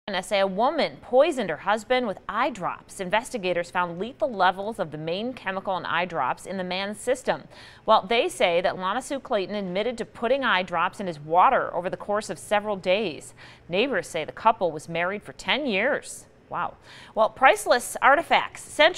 Woman allegedly kills husband with eye drops
Police in South Carolina says a woman poisoned her husband with eye drops. The couple was married for 10 years.